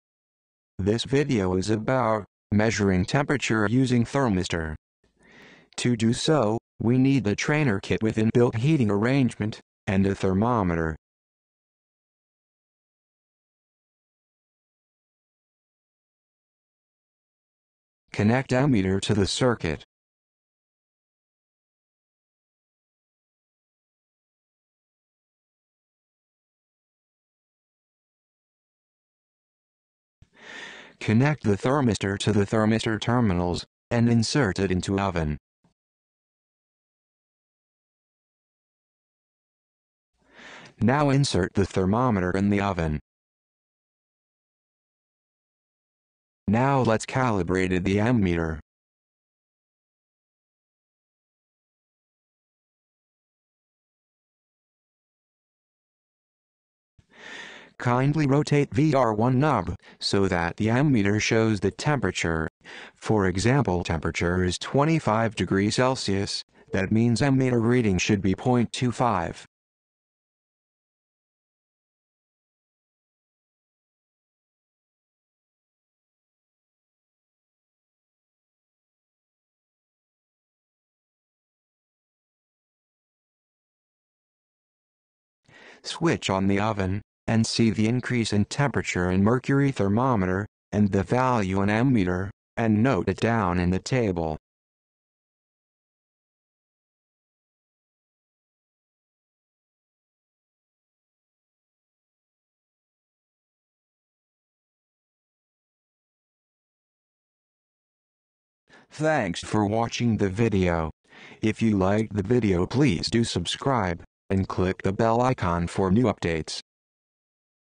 Temperature Measurement using Thermistor
OBJECTIVES:
To Study Temperature measurement Using Thermistor

APPARATUS: Temperature measurement Using Thermistor Trainer Kit , Connecting Wires (2).

Board has been designed specifically for understanding the Temperature Measurement technique using a Thermistor. The board is absolutely self contained and requires no other apparatus.